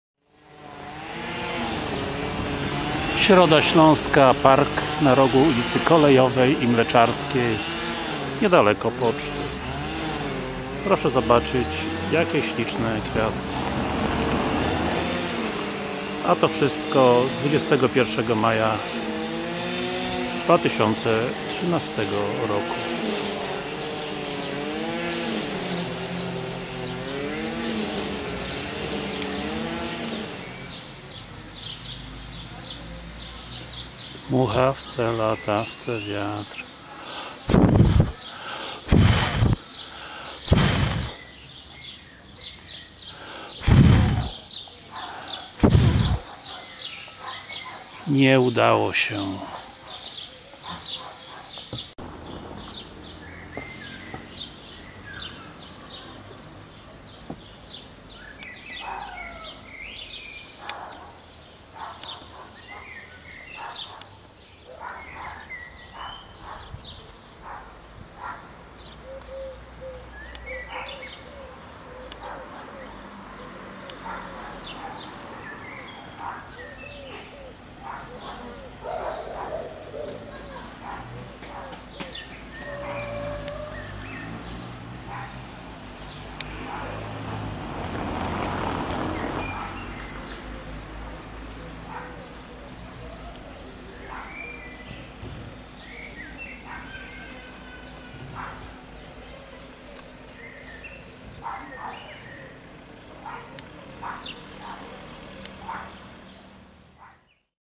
Środa Śląska park na rogu Mleczarskiej i Kolejowej
Park miejski w Środzie Śląskiej na rogu ulicy Kolejowej i Mleczarskiej sfilmowany podczas mojej wizyty w tej miejscowości 21 maja 2013 roku. Słychać odgłosy koszenia trawy. Niedaleko znajduje się miejscowy budynek Poczty Polskiej. Jest wyjątkowo ładnie, kwiatki kwitną a pszczóki ładnie latają